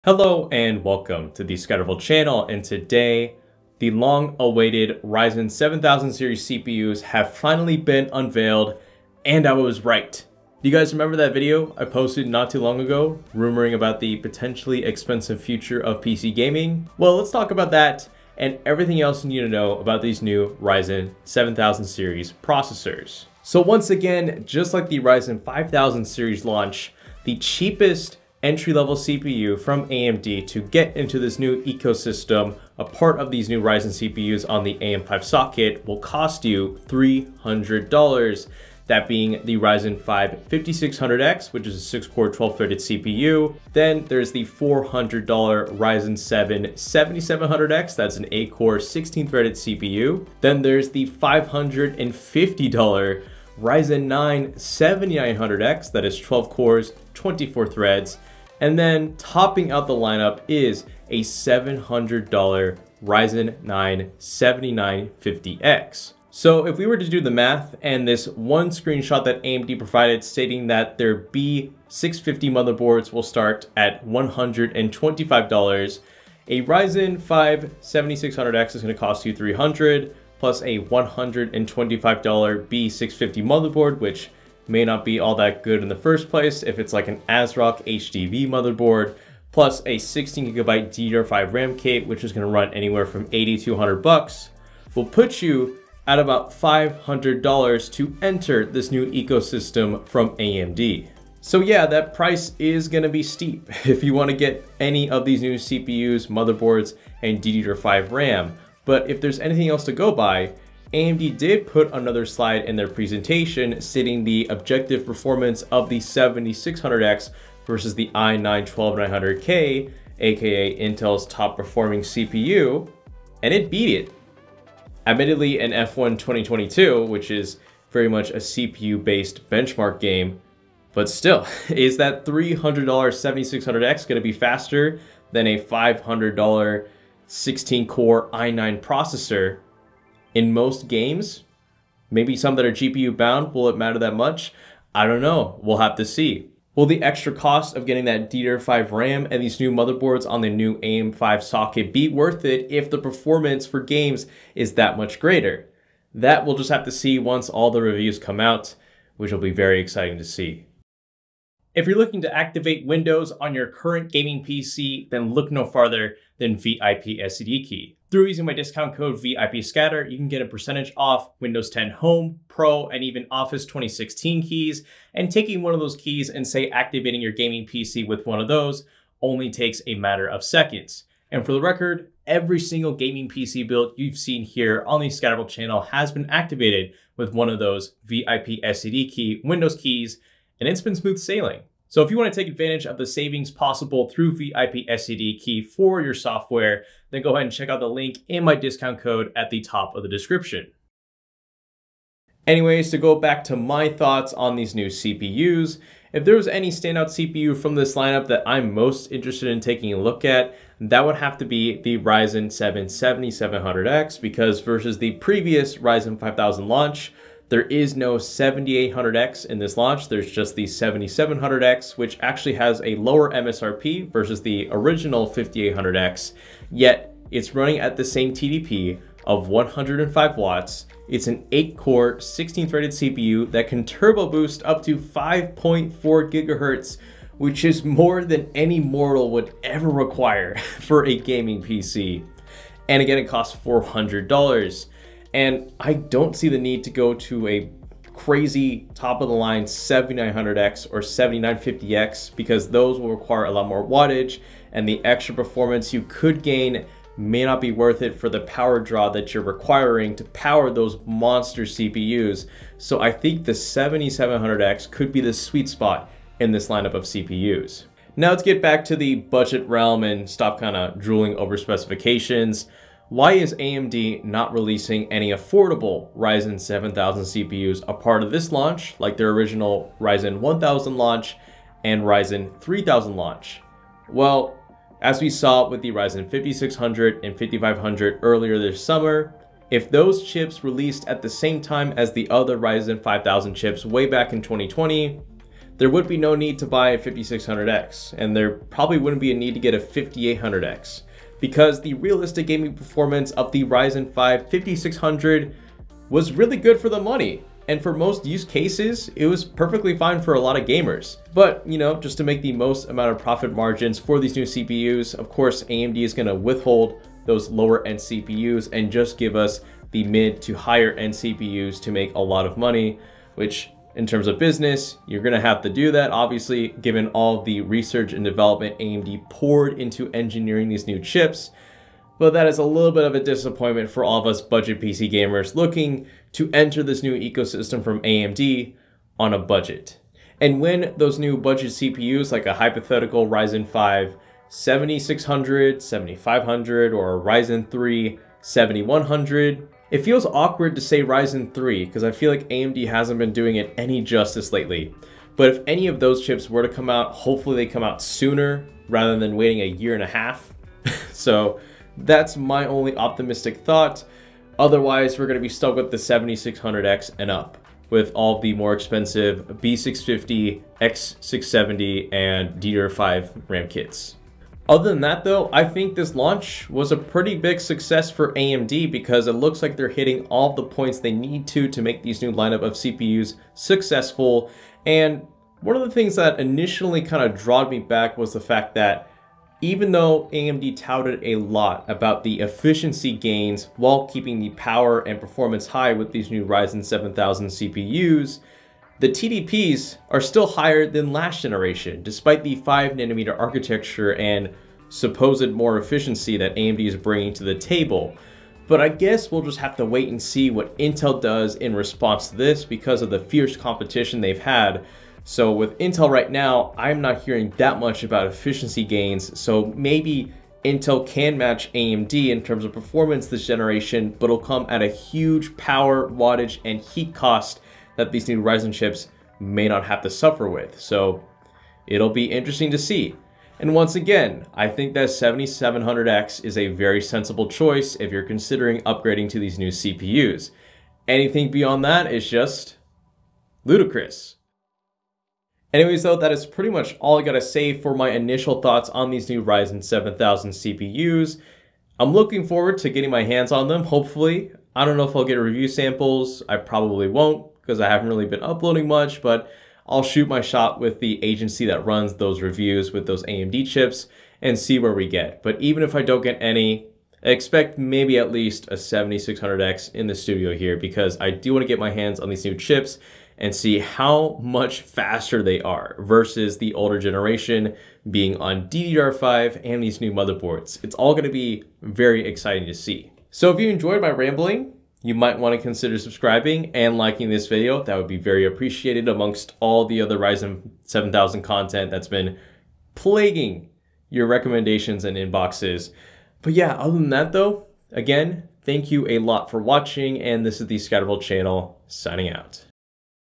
Where are the budget CPUs? - Ryzen 7000 CPUs Unveiled!
Here are my initial impressions and thoughts on the new Ryzen 7000 series CPUs releasing on September 27th!

0:00 - Hi!
0:26 - Pricing & Specifications
1:06 - ~$500 entry cost to enter Ryzen 7000
1:48 - Ryzen 5 7600X vs Intel Core i9 12900K
2:10 - Will Ryzen 7000 be worth the extra cost?
2:41 - Ad
3:22 - Ryzen 7 7700X looks spicy
4:28 - Where are the Ryzen 7100, 7200G, 7400G, 7500, and 7600?
6:14 - Final thoughts on Ryzen 7000
6:48 - Ryzen 7000 vs Intel 13th Gen Raptor Lake
7:26 - Thank you for watching